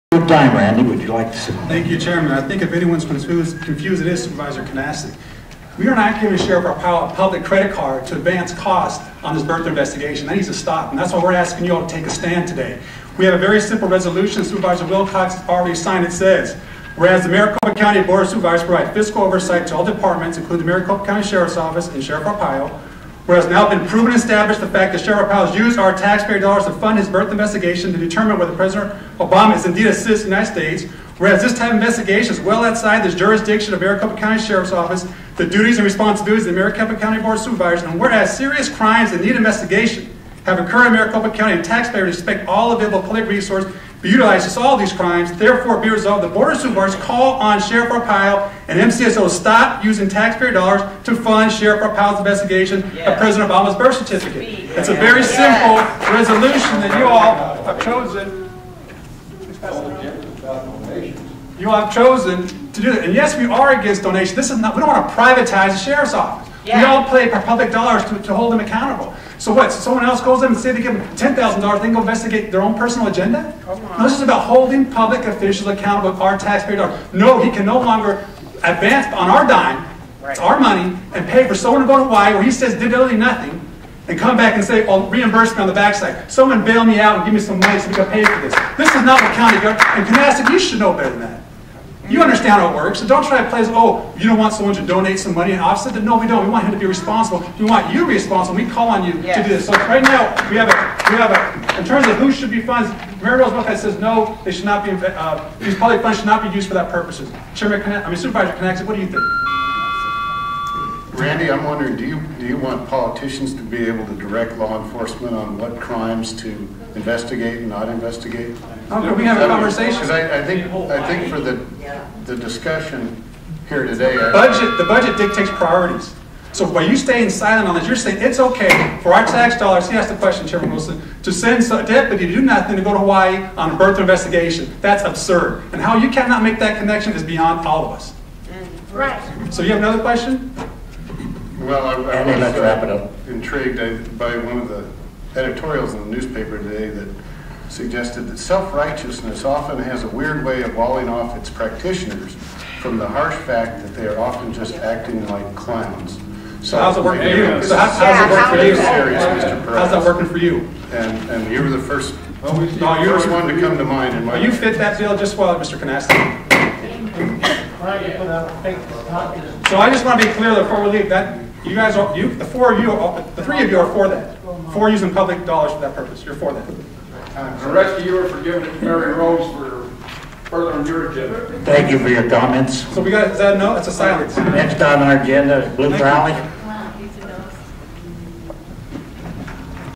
Supervisor Andy Kunasek and Randy Parraz Spar Over Sheriff Joe Donation!
Raw footage: The Maricopa County Board of Supervisors on Wednesday rejected by a tie vote $5,000 in private donations intended to help cover travel expenses for the sheriff's cold-case posse as it investigated President Barack Obama's birth certificate -- the so-called "birther" probe.